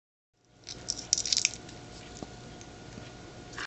Gold Plated Metal Pistachio Opener
Lightweight attractive Gold Plated Metal Pistachio Opener tool. An exclusive gift for that hard to buy for special person. How to open pistachios.